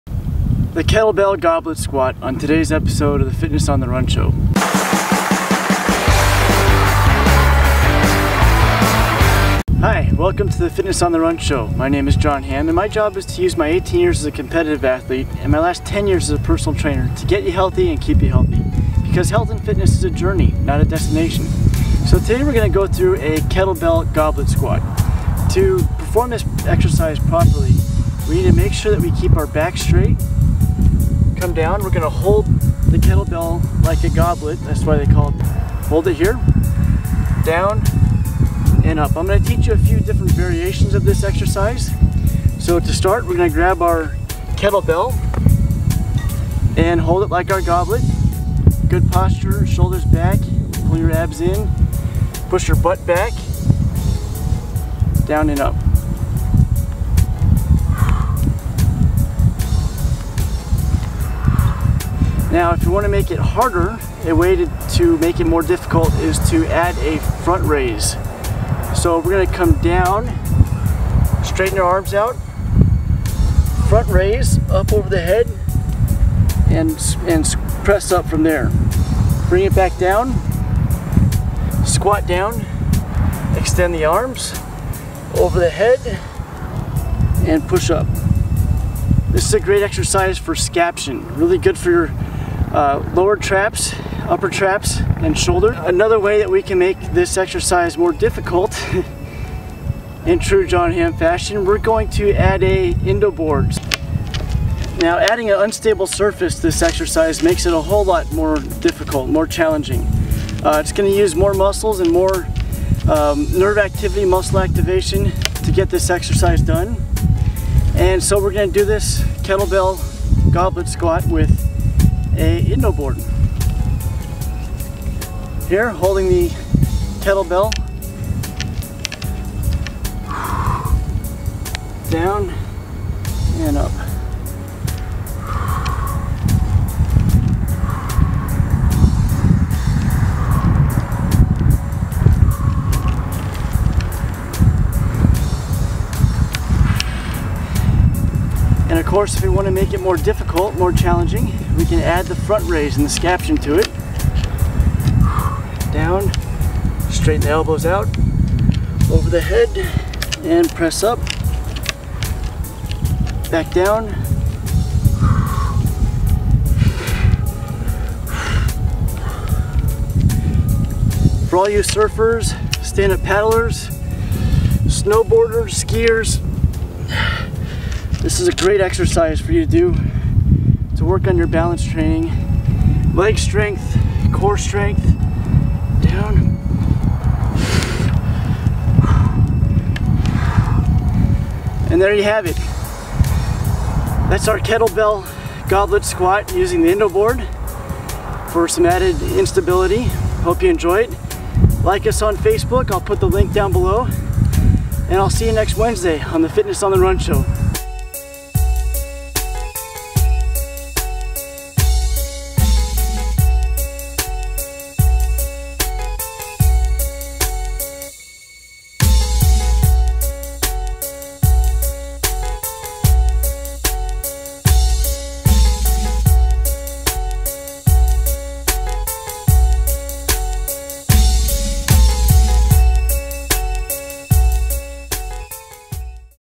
Kettlebell Goblet Squat - Fitness On The Run Show S1E6
The Kettlebell Goblet Squat is a great exercise for making gains in your squatting abilities, leg strength, core strength, and when using the Indo Board, balance training.  Here, Jon goes through a progression of the Goblet Squat first with a kettlebell, then performing a kettlebell front raise while squatting.  To make it a balance training exercise, use an unstable surface like the Indo Board to increase proprioception and muscle activation without an increased load of weight.  To make it even more challenging, add a kettlebell front raise while on the Indo Board.  Remember, while on the indo board, floor surface is very important.  Doing this on a harder surface will make it quicker, a thicker surface like grass will slow down the board's movement and make it easier to balance.